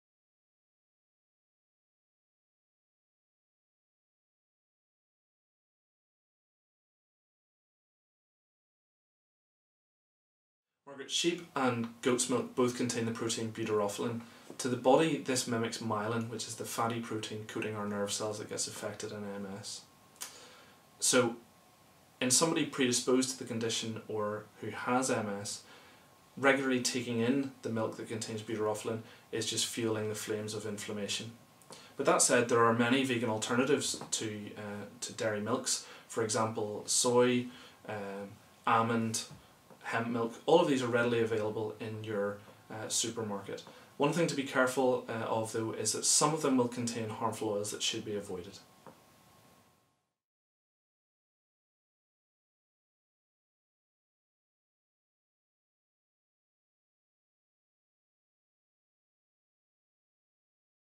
Is Sheep's Or Goat's Milk Suitable For Someone With Multiple Sclerosis?
Dr Jonathan White answers some frequently asked questions about the OMS Recovery Program, an evidence-based program that has been shown to benefit people with multiple sclerosis. The rest of the Q&A can be found in this playlist.

Dr Jonathan White will be speaking at our Belfast event taking place on the 4th of March.Get your tickets here: